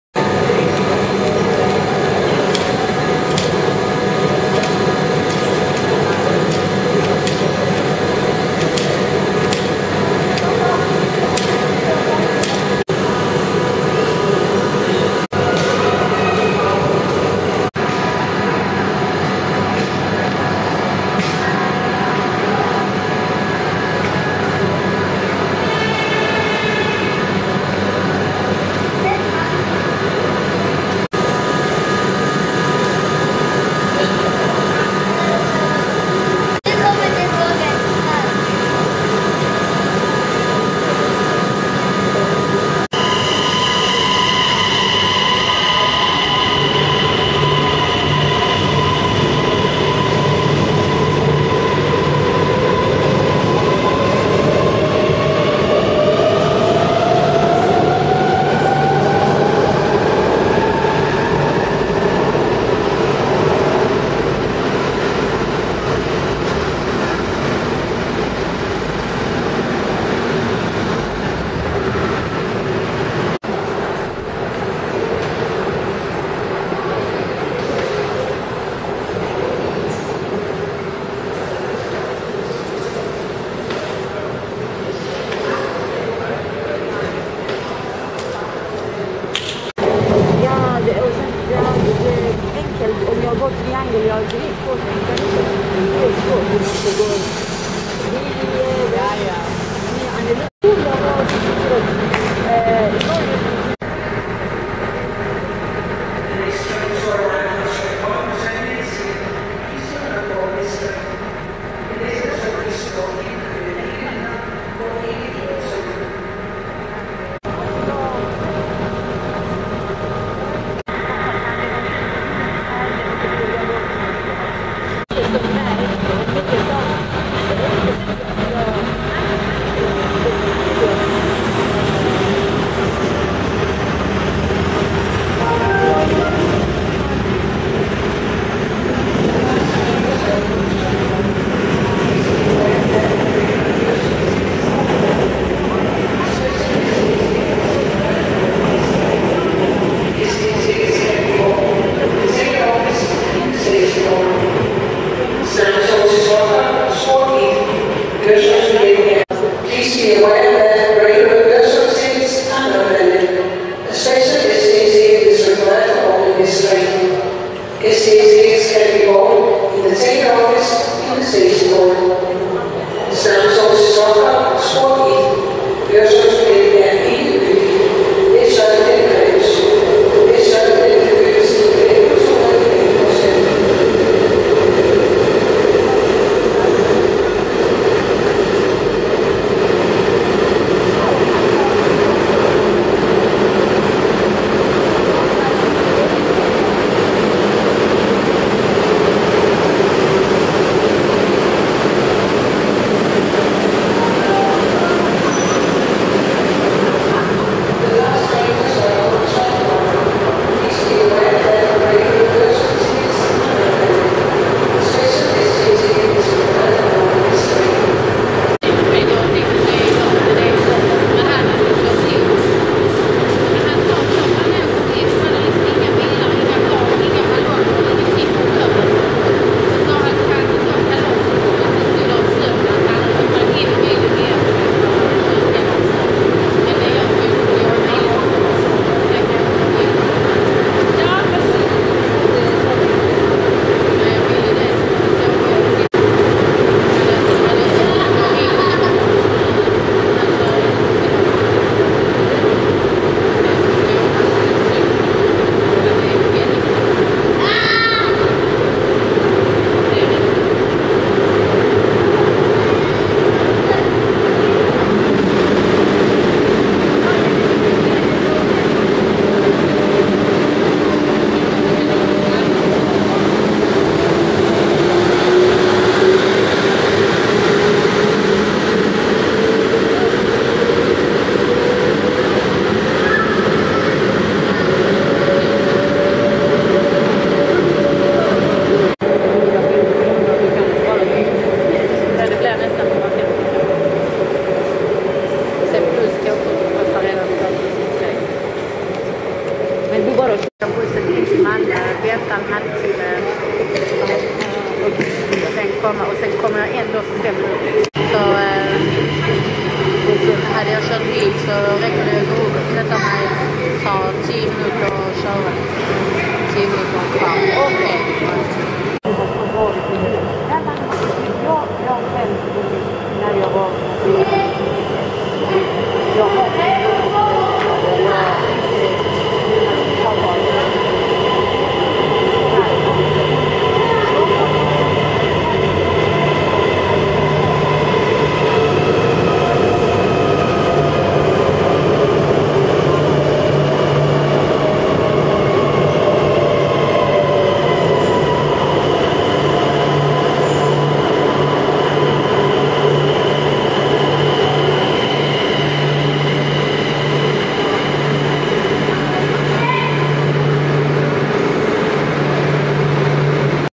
Trains at Kastrup C
Kastrup airport train station, Copenhagen, Denmark: Trains and people are on the ground, love and airplanes are in the air.